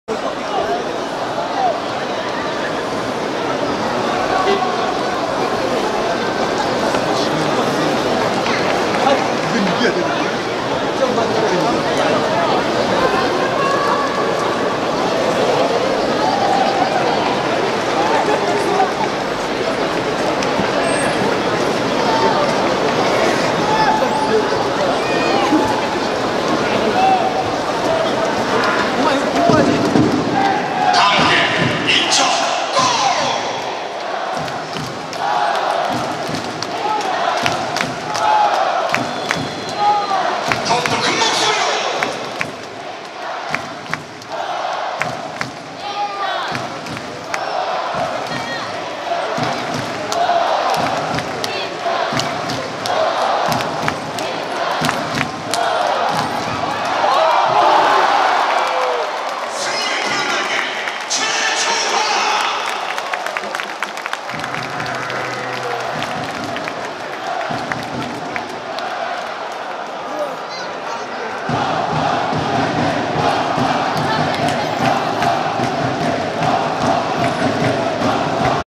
[인모것직캠] 170708 po조현우wer 인천유나이티드(0) vs 대구FC(0)
170708 인천유나이티드(0) vs 대구FC(0)

말이 필요없는 국가대표 골키퍼
대구FC의 조현우의 선방장면

p.s 진짜 잘한다.